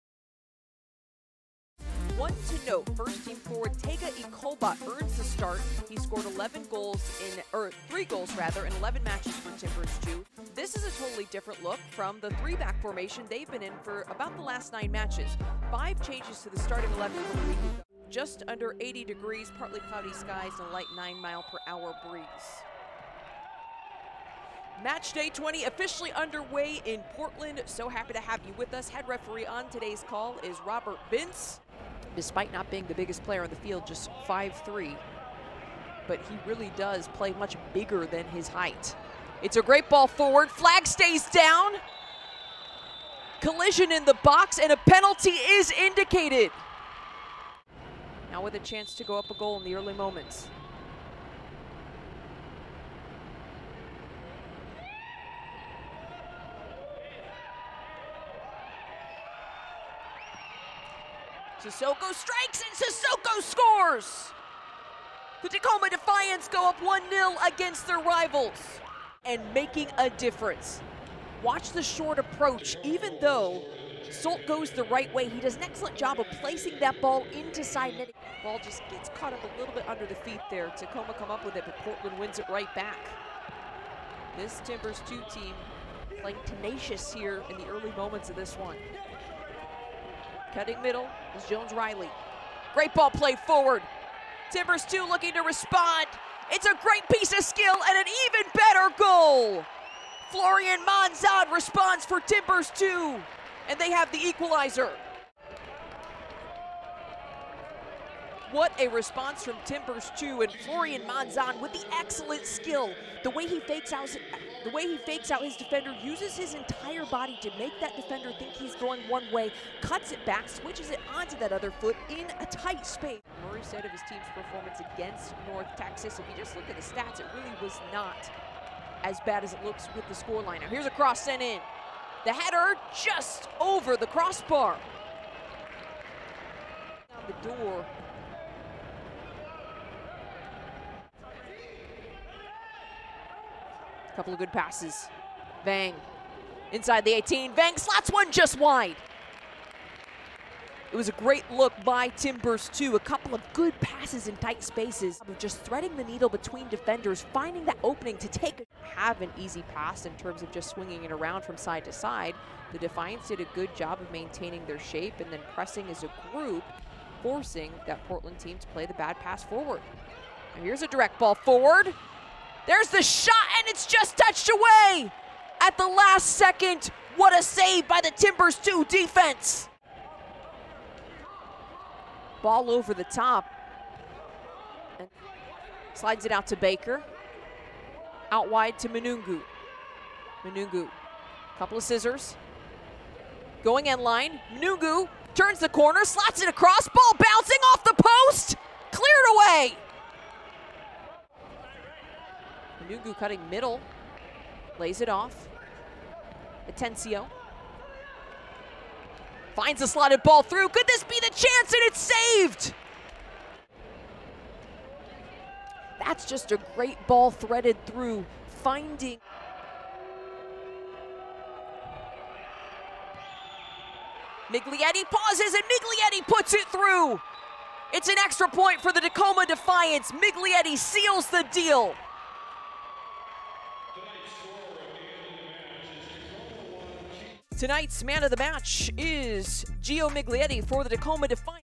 HIGHLIGHTS | Timbers2 vs. Tacoma Defiance | August 05, 2023
Forward Florian Monzón scored his team-leading seventh goal of the season in a 1-1 draw with rival Tacoma Defiance at Providence Park.